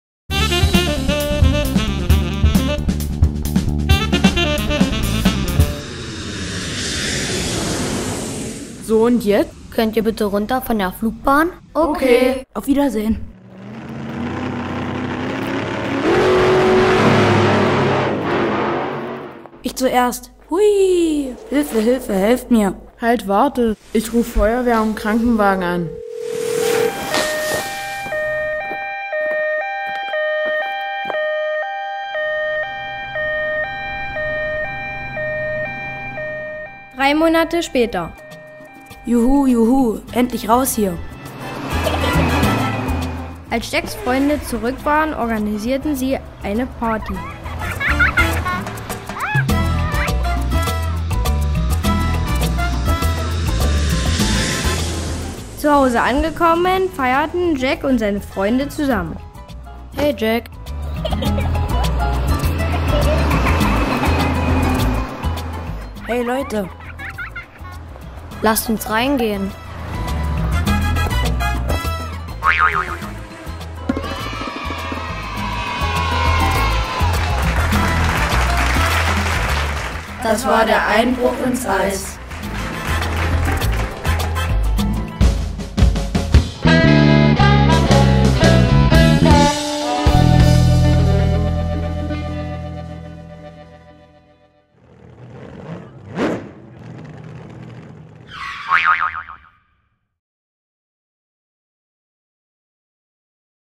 Der Eisbruch ins Eis
Ein Film von Florian, Mitchell, Kai und Chris über Freundschaft auch in schwierigen Momenten. Produziert von SchülerInnen der 6. Klasse aus der Sekundarschule in Annaburg.